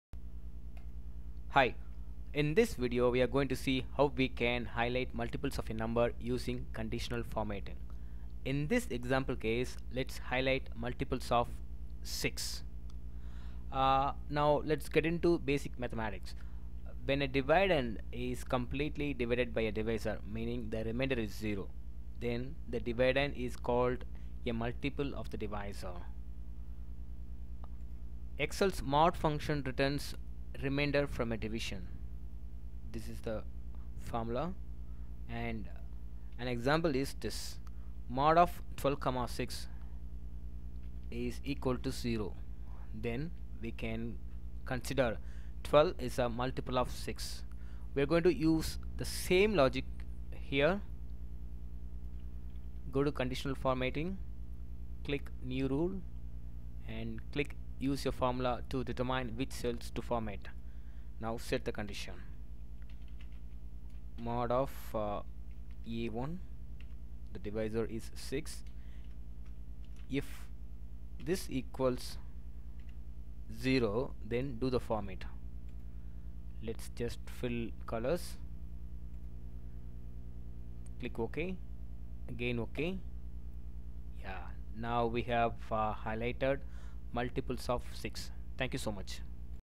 How to highlight multiples of a number using conditional formatting in excel?
Highlighting multiples of a number using conditional formatting in excel